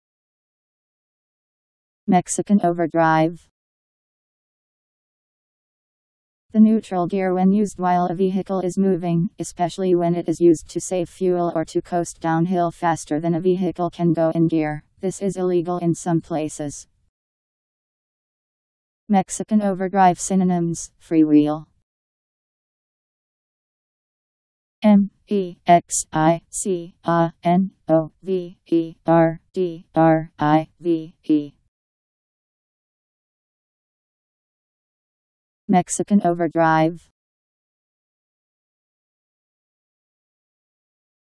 Mexican overdrive Meaning
Video shows what Mexican overdrive means. The neutral gear when used while a vehicle is moving, especially when it is used to save fuel or to coast down hill faster than a vehicle can go in gear. (This is illegal in some places.). Mexican overdrive synonyms: freewheel.  Mexican overdrive Meaning. How to pronounce, definition audio dictionary. How to say Mexican overdrive. Made with MaryTTS and Wiktionary